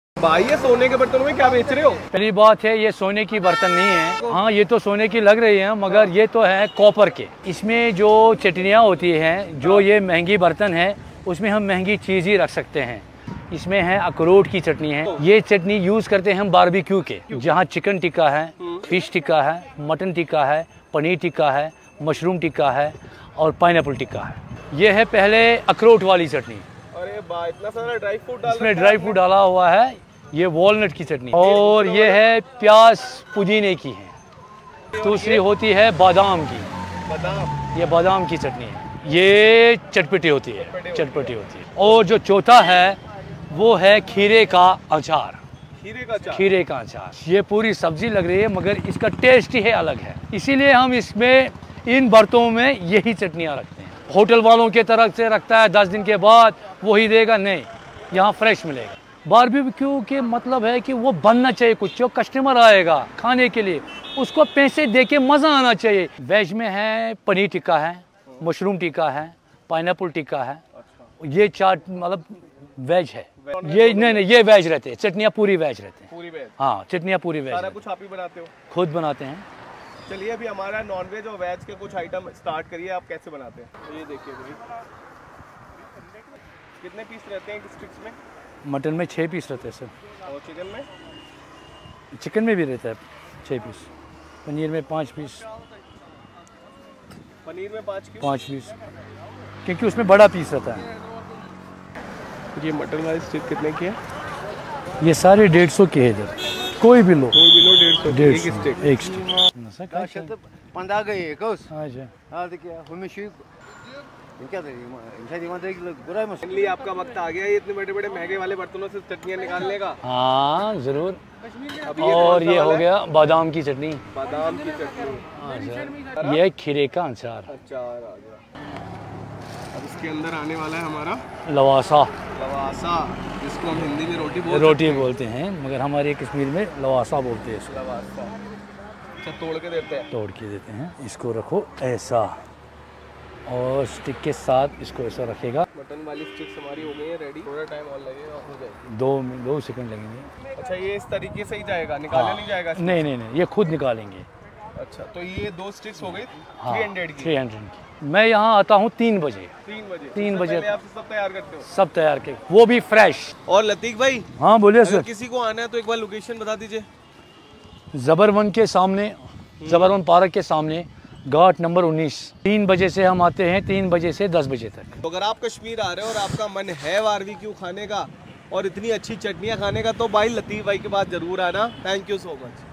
सोने के बर्तन मे खिलाते है चटनी ||Lateef Barbeque Kashmir Food
The kashmir  food tour Lateef Barbeque amezing food concept  sone ke bartan me rakhte hai chatniya.